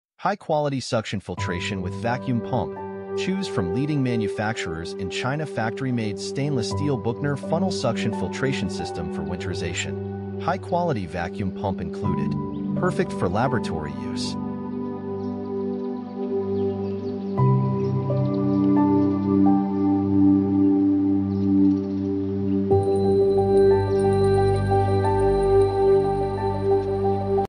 High-Quality Suction Filtration with Vacuum Pump - Choose from Leading Manufacturers in China
High-Quality Suction Filtration with Vacuum Pump - Choose from Leading Manufacturers in China Factory-made Stainless Steel Buchner Funnel Suction Filtration System for Winterization. High-quality vacuum pump included. Perfect for laboratory use.